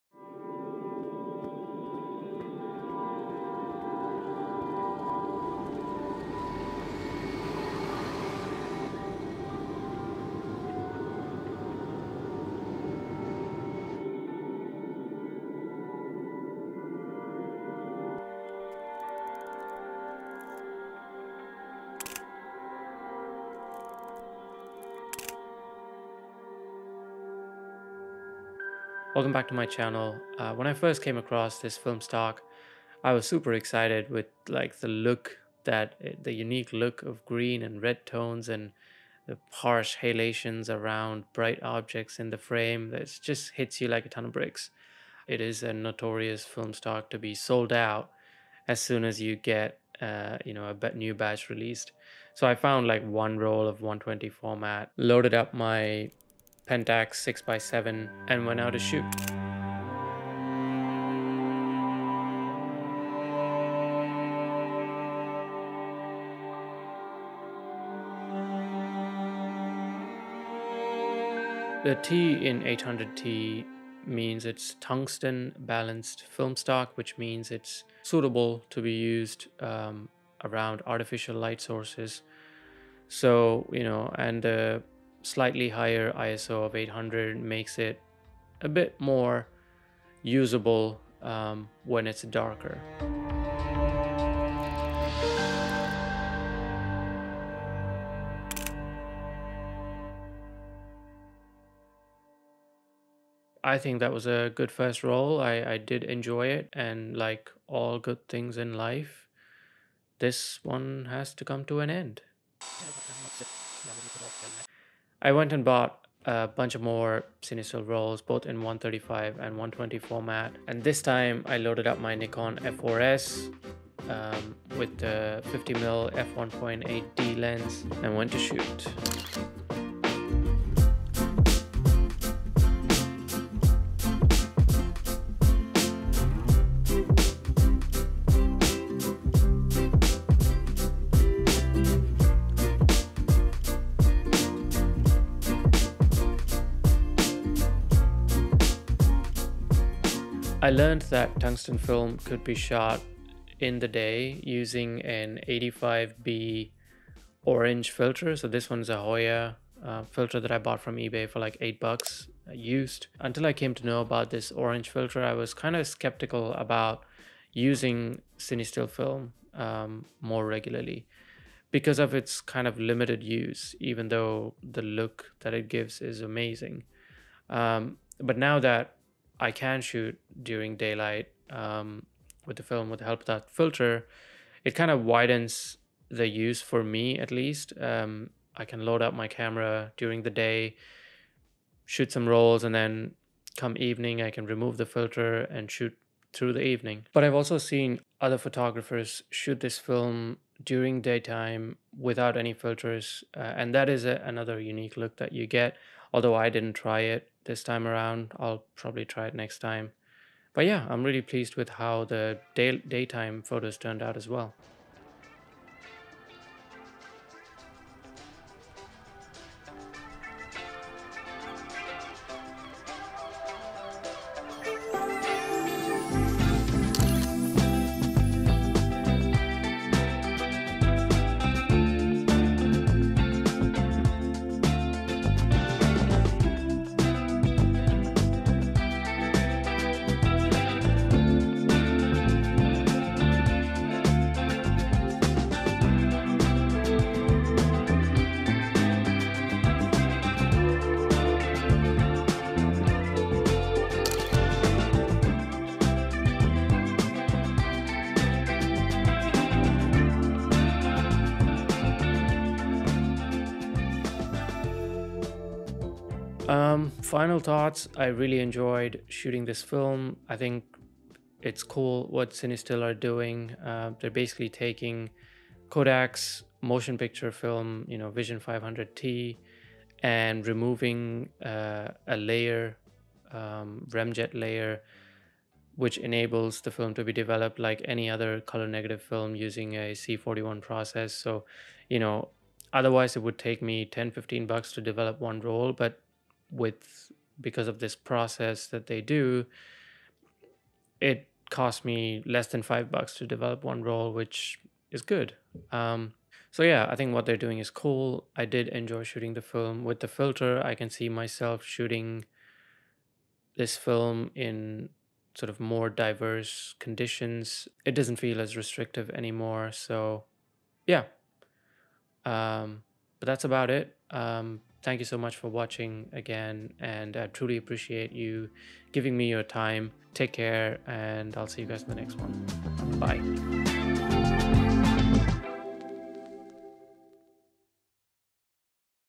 Shooting Cinestill 800T in London
A few weeks ago, I had the pleasure of trying out CineStill 800T for the first time. It was foggy in Canary Wharf, perfect weather for this film stock. Soon after I got them developed, I had to shoot more. In this video, I share my experience shooting it and some images from those two rolls.

Chapters
00:00 Intro
00:59 Images
01:11 800T Talk
01:44 Round 2
02:09 Night shots
02:30 85B Filter
03:35 Day Shots
04:26 Final thoughts